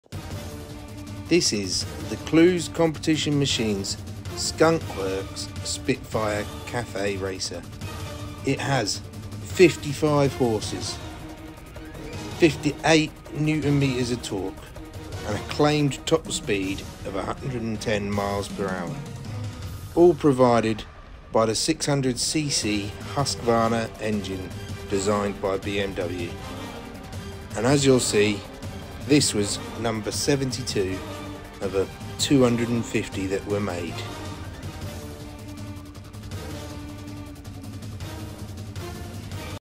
CCM Spitfire Specs and walk around.
⁠@ZionAutomotiveLogistics collected this fantastic look CCM Spitfire Cafe Racer by Skunkworx a few weeks ago for ​⁠@chrishallmotorcycles but I wanted to do a more in depth walk around with some of its specifications as I who doesn’t like to know them!

It’s  by Clews competition machines (CCM) and was a limited edition Cafe Racer done by Skunkworx.

It has 55 bhp
58 nm torque
With a top speed claimed of 110mph.

And this particular one was no 72 of a possible 250.